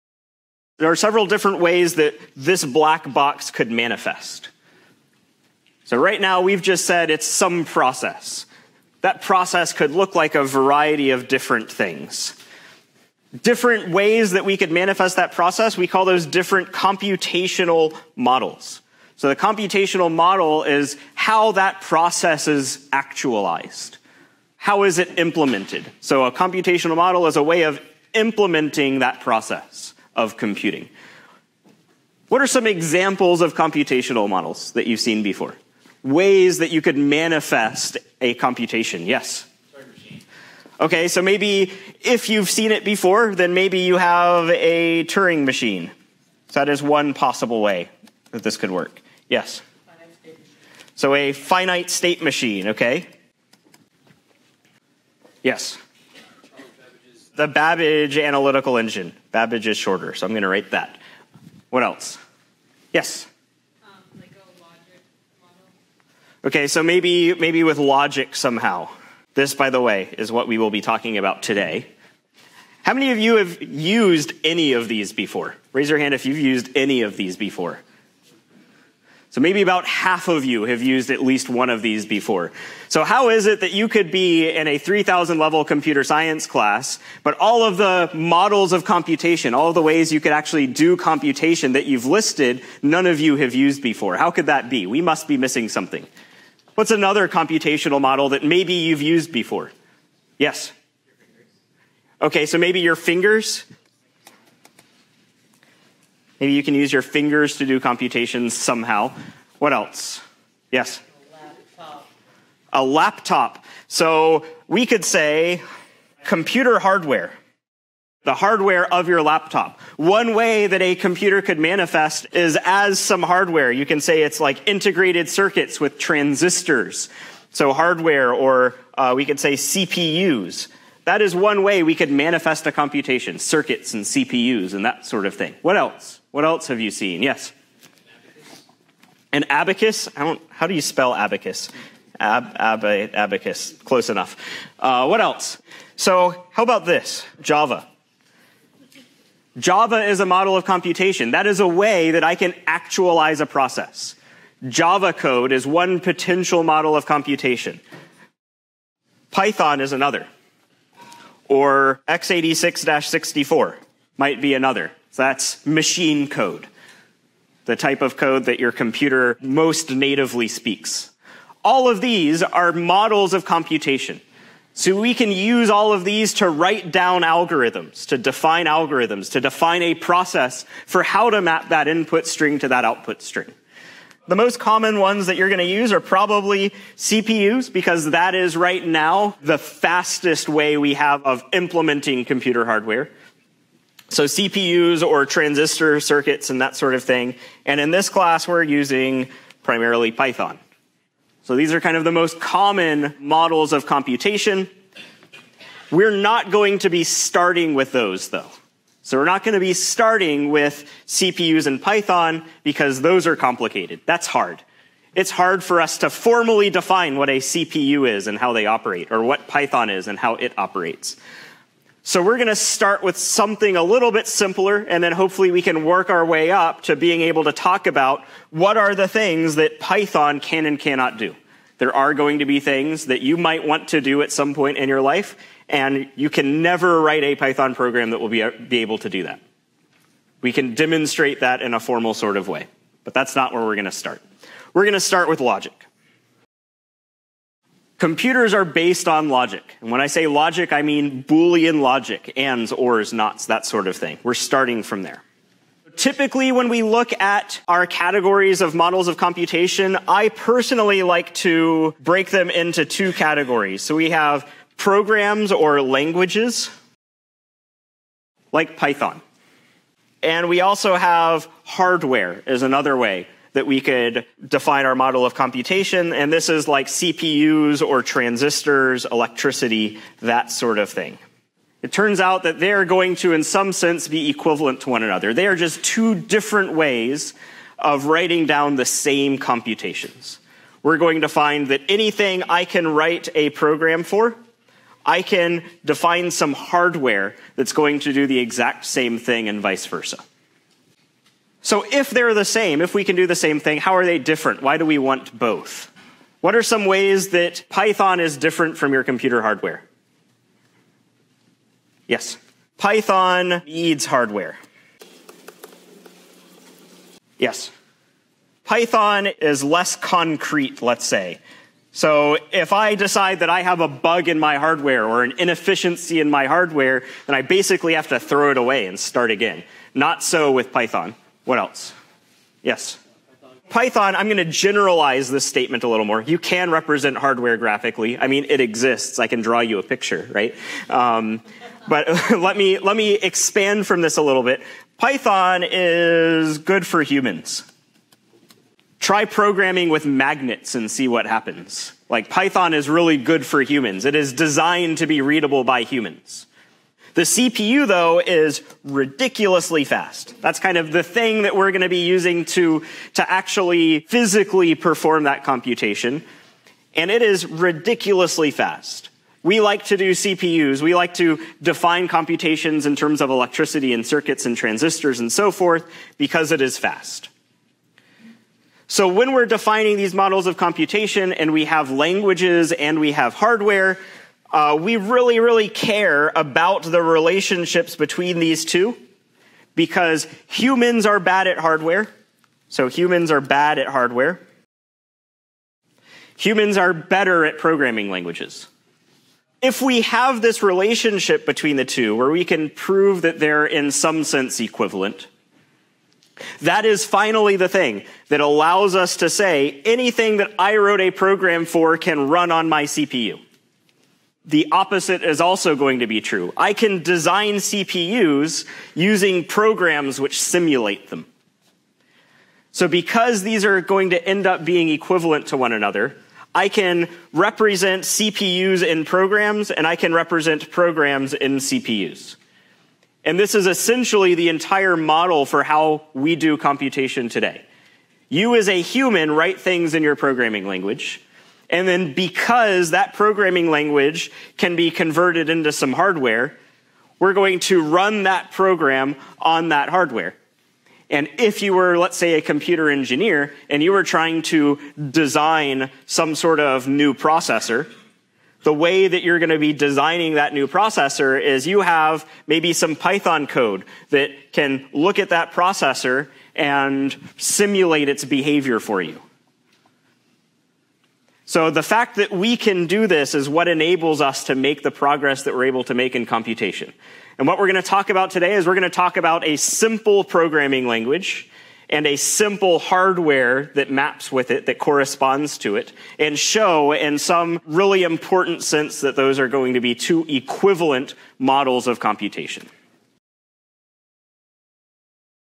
Modeling Computers
5.2: Modeling Computers
- How should we model computing?
- Programs and Hardware

Nathan Brunelle and David Evans